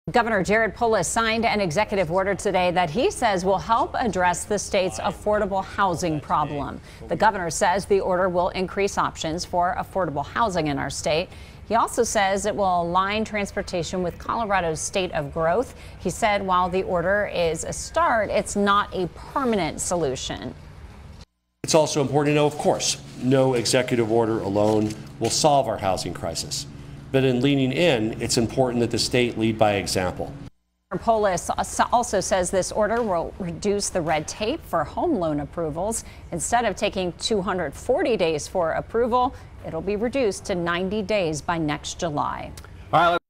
Governor Polis signs executive order to address affordable housing problem
During today's press conference, Polis said that while this order is a start, it is not a permanent solution.